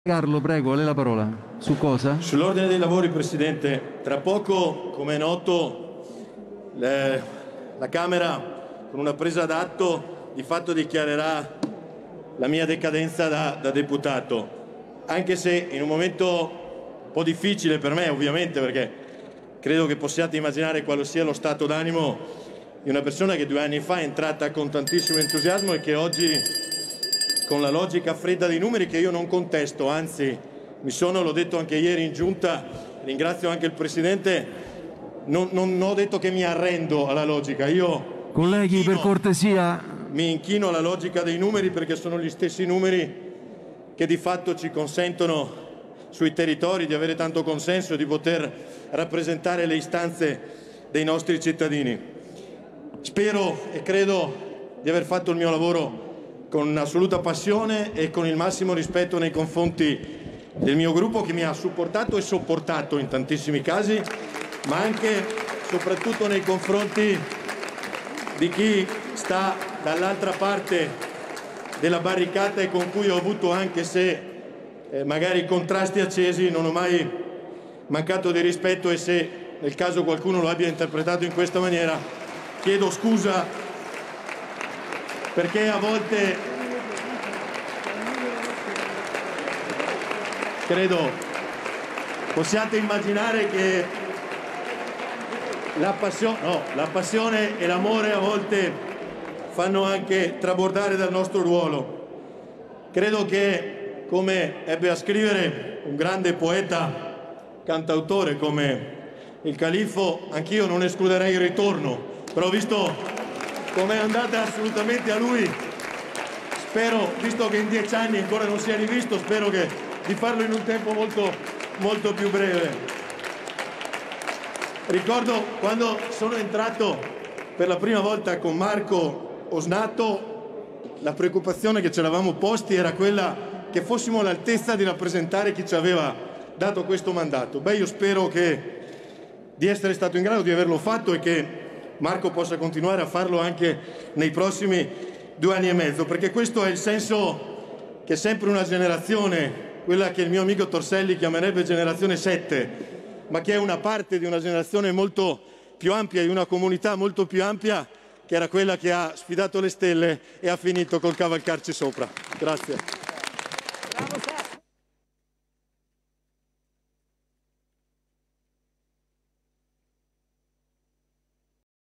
Il saluto di Luca De Carlo alla Camera dei Deputati
🔴 Tra poco, come noto, la camera dichiarerà la mia decadenza da deputato. Mi inchino alla logica dei numeri perché sono gli stessi che ci consentono sui territori di avere tanto consenso e di poter rappresentare le istanze dei cittadini. Spero e credo di aver fatto il mio lavoro con assoluta passione e con il massimo rispetto nei confronti del mio gruppo che mi ha sempre supportato e nei confronti di chi sta dall’altra parte della barricata. Quando sono entrato alla Camera dei deputati per la prima volta, la mia preoccupazione era essere all’altezza di rappresentare chi mi aveva dato questo mandato, spero di averlo svolto bene

Il saluto di LucaDeCarlo alla Camera dei Deputati